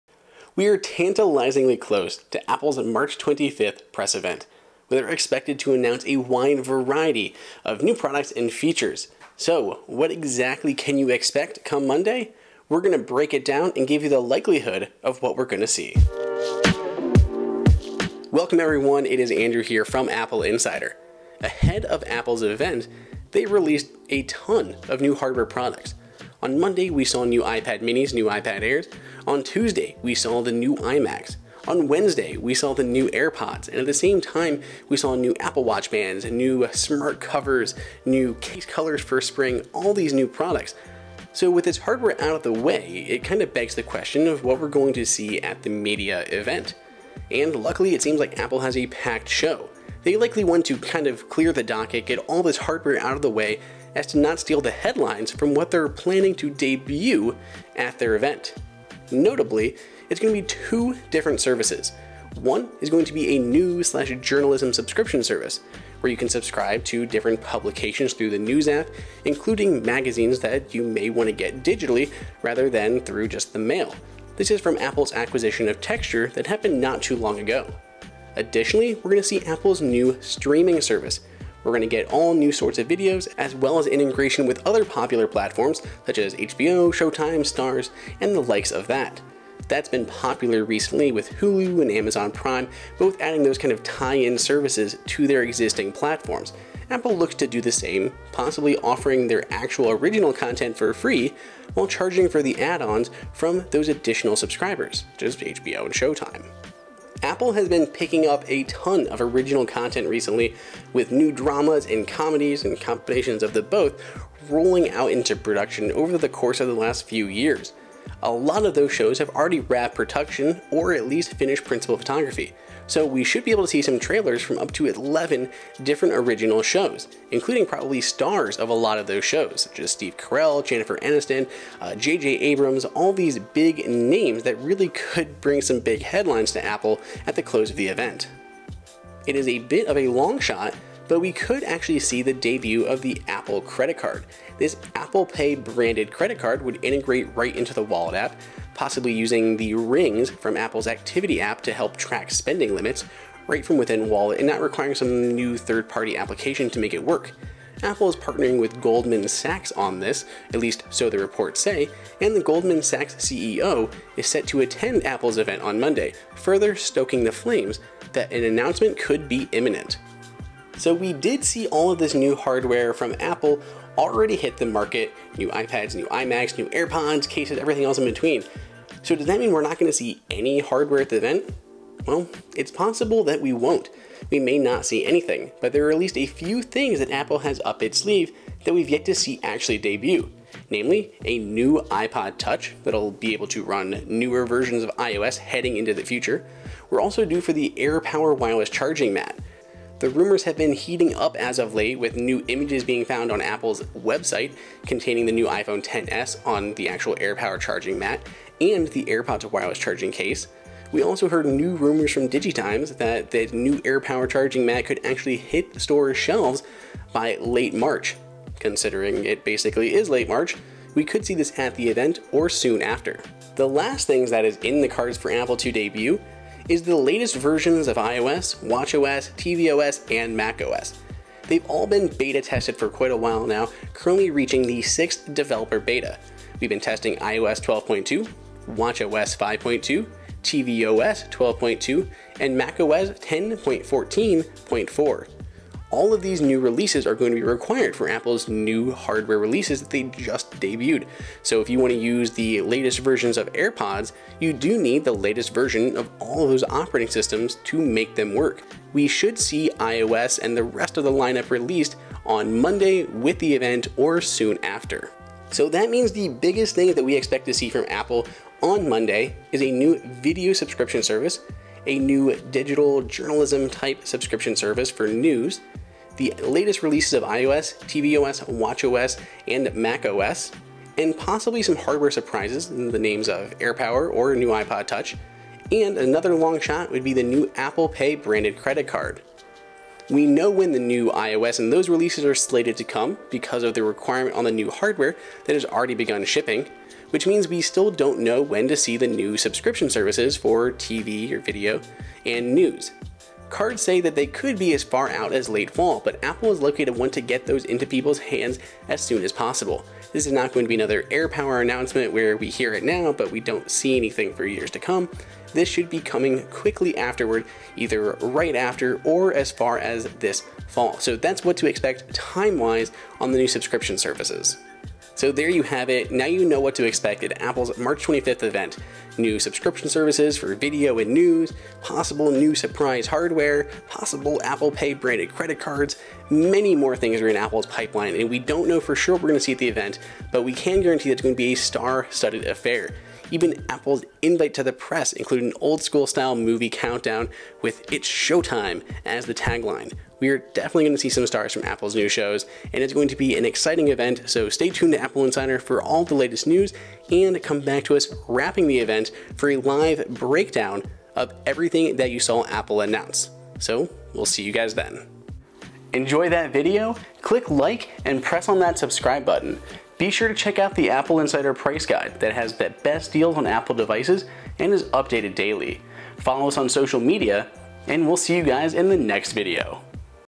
Everything To Expect From Apple's March 25th "Show Time" Event!
Get an early look at what to expect during Apple's March 25th media event at the Steve Jobs Theater!